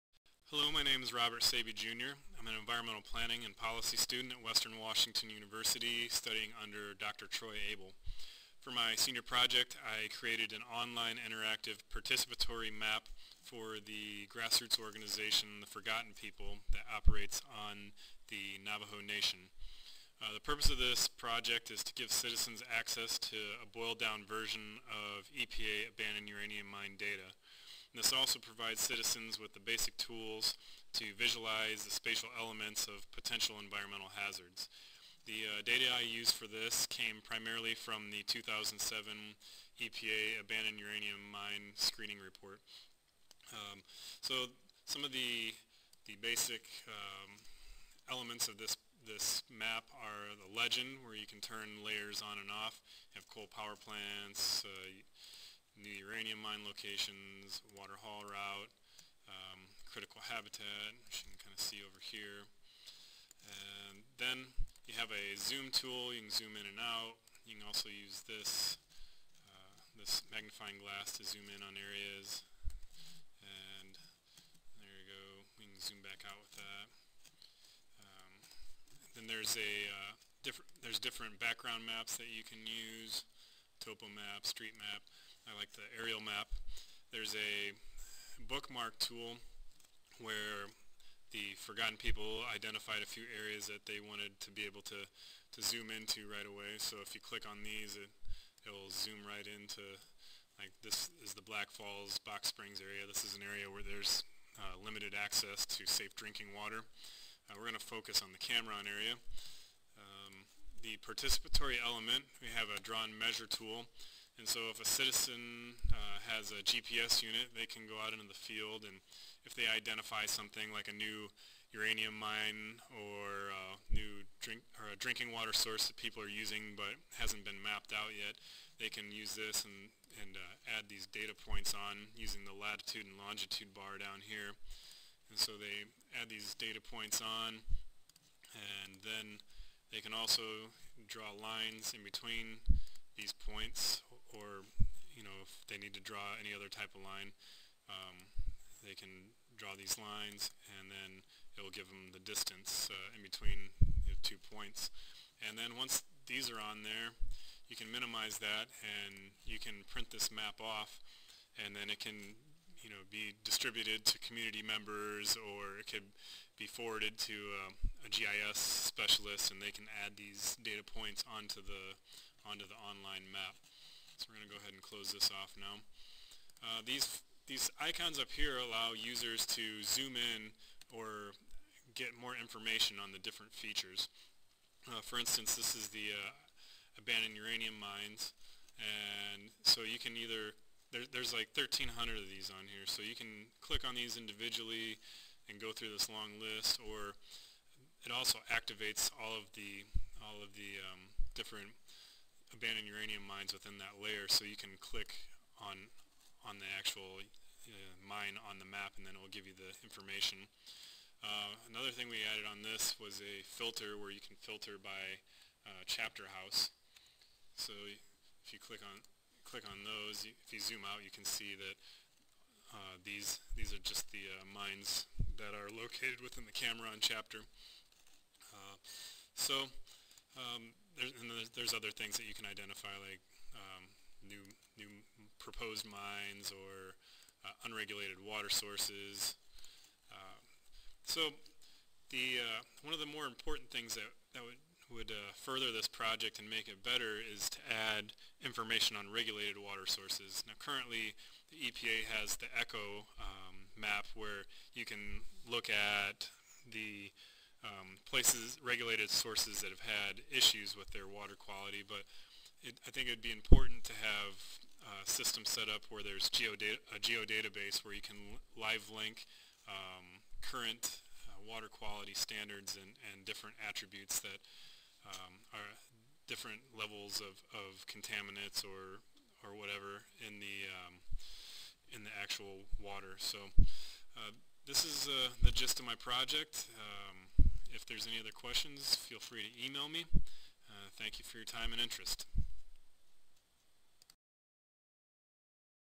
ForgottenPeople_Participatory_Mapping.avi
The online map developed in this project uses data from the EPA 2007 Abandon Uranium Mines and the Navajo Nation: Atlas with Geospatial Data to give citizens access to basic information on unregulated water sources and abandon urnanium mine features.  The map also provides citizens with the basic tools to visulize the spatial elements of potential environmental hazards.

 Environmental Justice is a relatively new field for environmental advocacy.  One the many attributes that is illustrative of environmental injustice is proximity to pollution.  Developments in Geographic Information Systems (GIS) and the gathering of spatial data have furthered the implications of environmental justice.  The GIS technical expertise is not always available to grassroots organizations and thus the spatial nexus is sometimes missing in the struggle for justice.   This project was designed to assist the Navajo grassroots organization The Forgotten People in both policy development and participatory mapping.